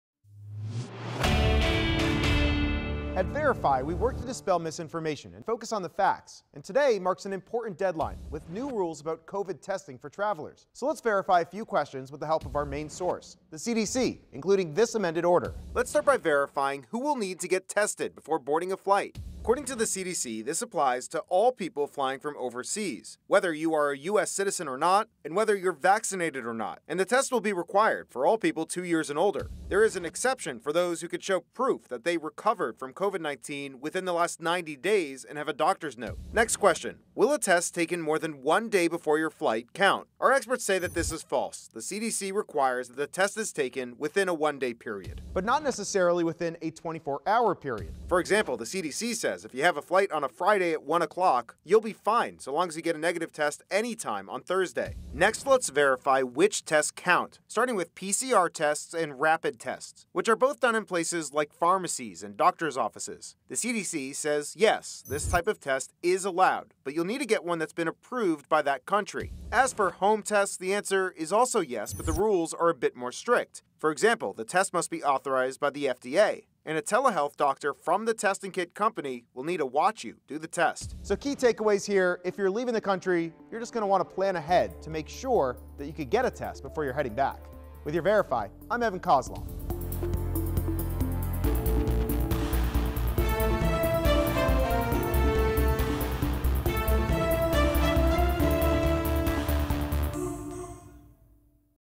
VERIFY: Breaking Down New COVID Testing Rules For Flying
International travelers will now have to get tested one day before heading to the states.